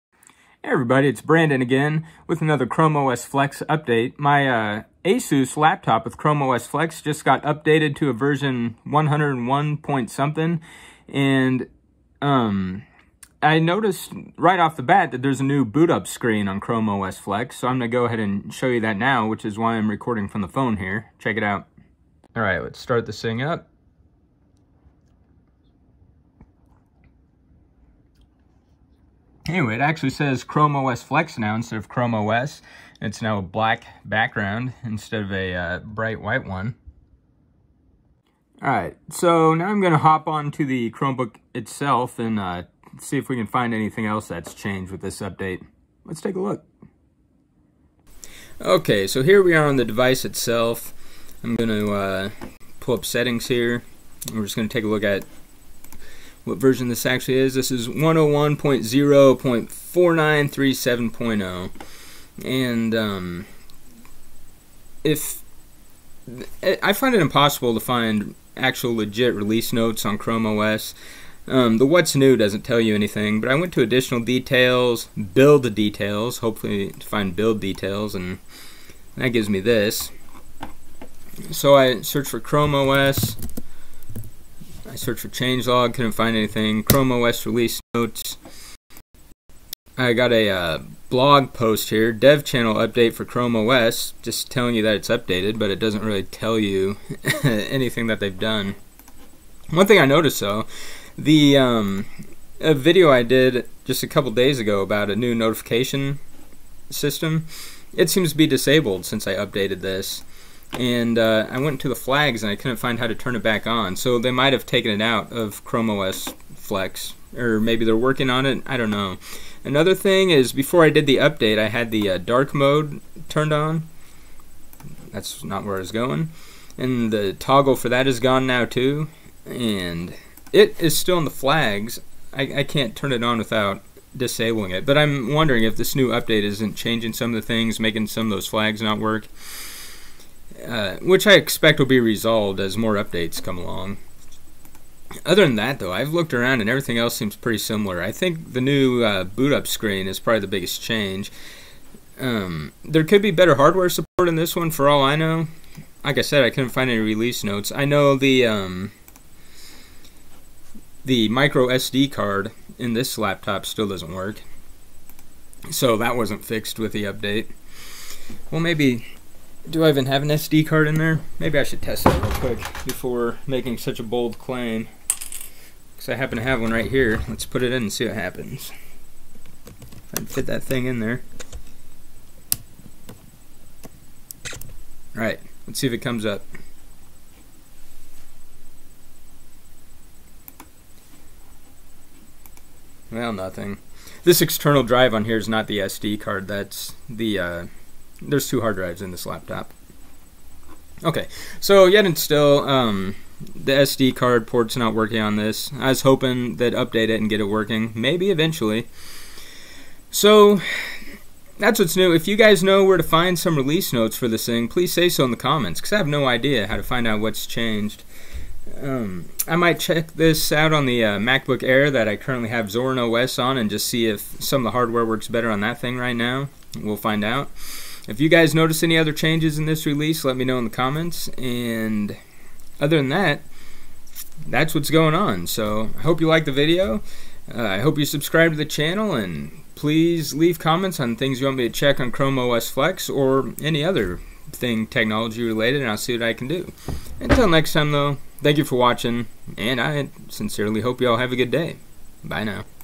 Chrome OS Flex Updated to v101.0.4937.0
Checking out what's new on the latest update to Chrome OS Flex, which includes a snazzy new boot screen that actually now says Chrome OS Flex on it.